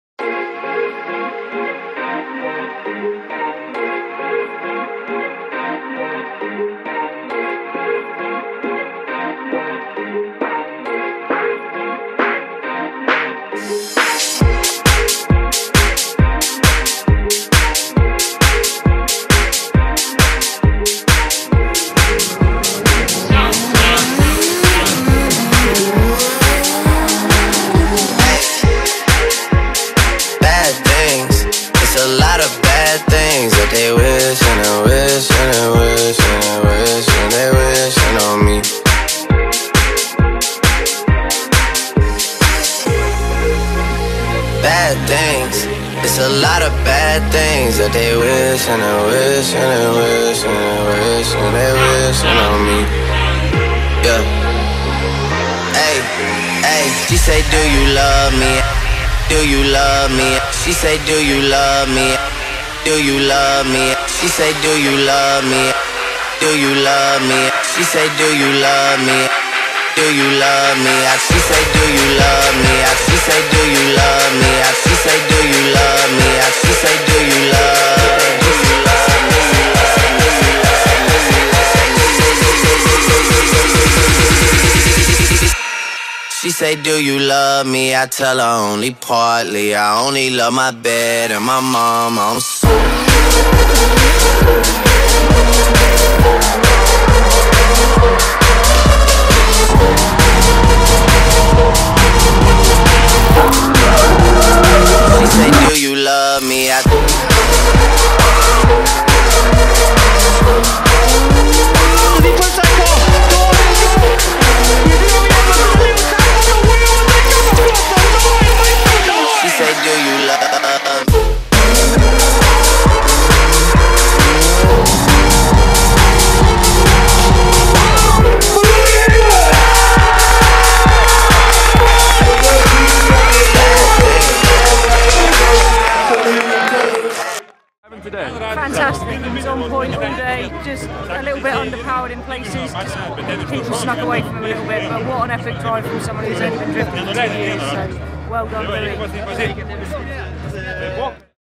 Lwi Edwards, #83 Pro Am British Drift Championship.
Footage of the up and coming drifting star, Lwi Edwards of Lwi Motorsport from Round five of the 2019 British Drift Championship where he finished third in the Pro am's which enabled him to progree to the pro's the following day. Our young cameraman Jordan Clift filmed and edited the footage.